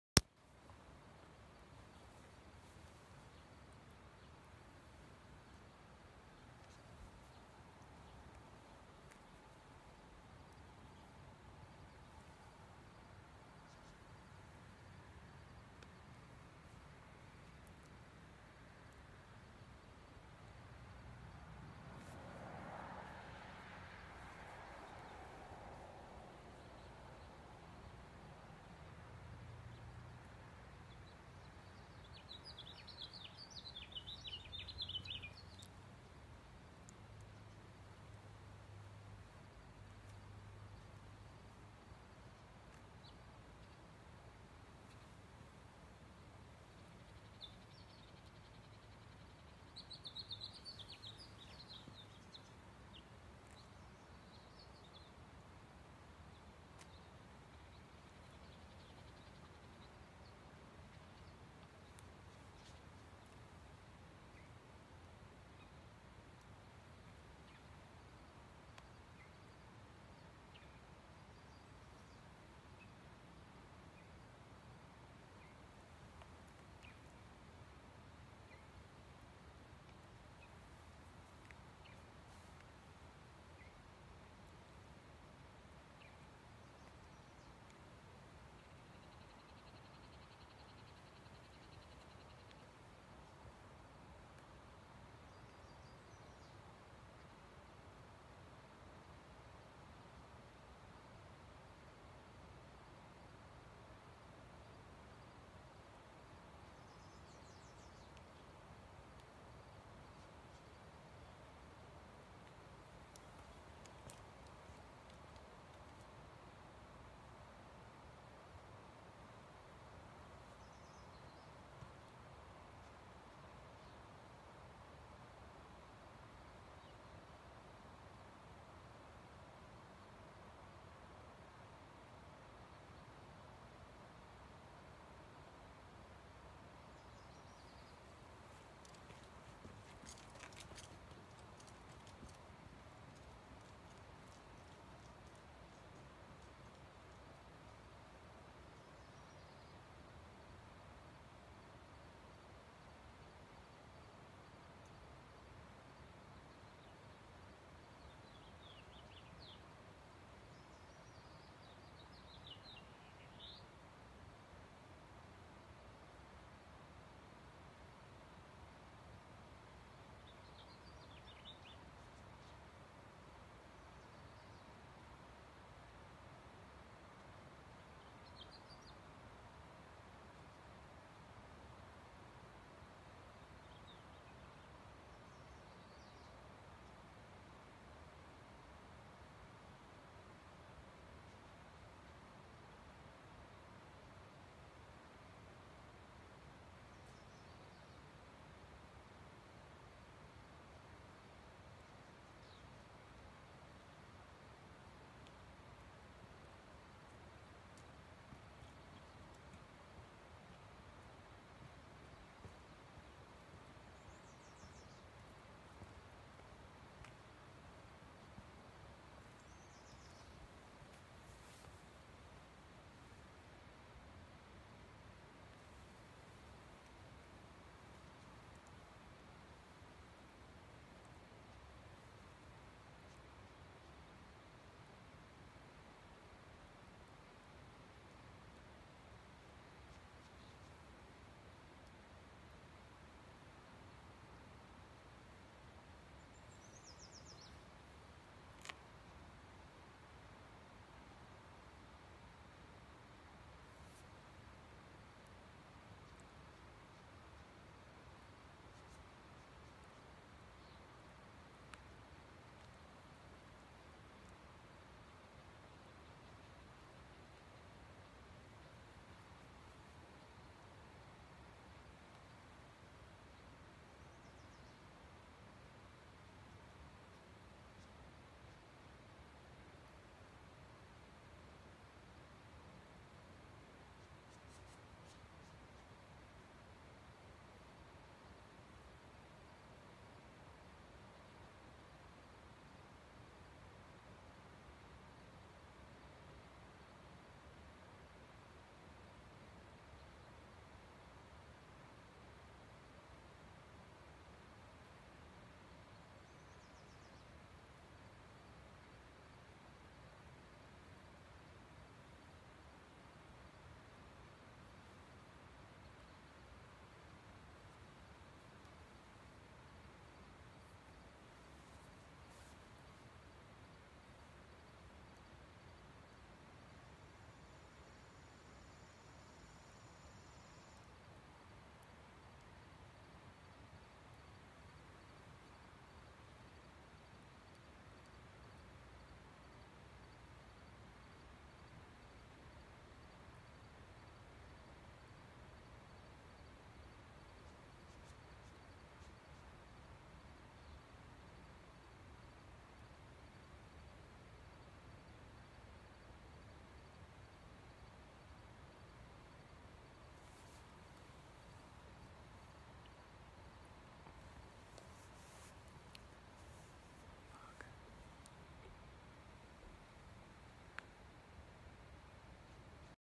Sunset Encore (4/26/20)
The hazy clouds might have blocked the sun on the cliffs, but they put on their own show after sunset. The light was too good to stop on April 26, 2020.